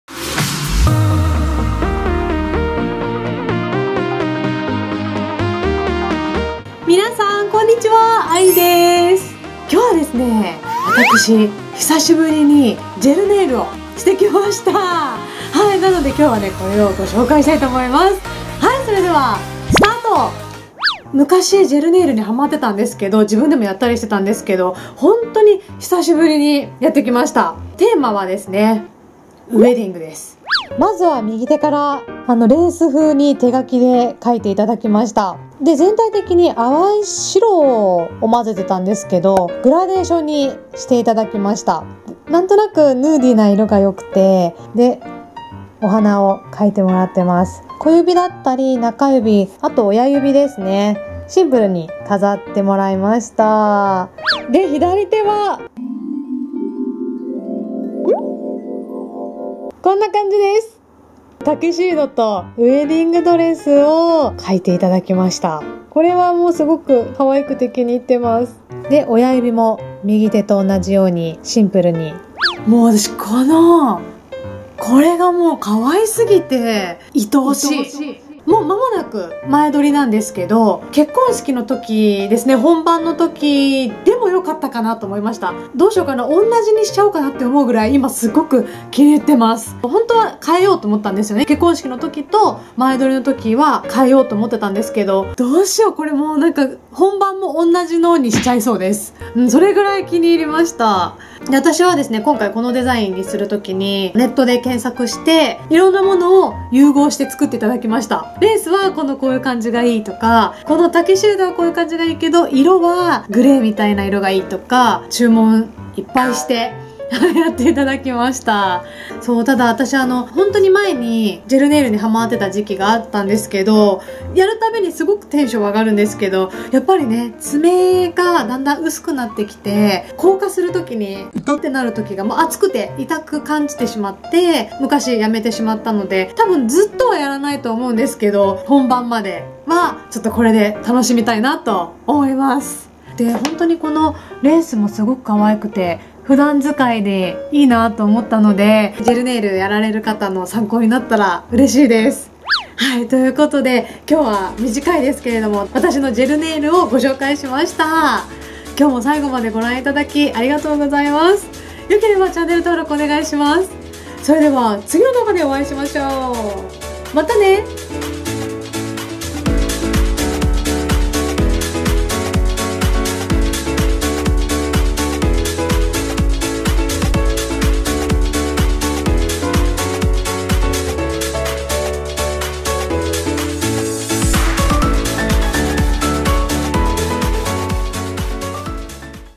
可愛すぎるウェディングネイル紹介！《My wedding nails》byアラフォー
まつエクをやった。ネイルもやった。
あとは痩せるだけ・・・。

今日もあなたがハッピーでありますように♡

♡あいりのサブチャンネル

〈不慣れで気まぐれな呟き〉

♡レター、贈り物等の送り先はこちら
あいり宛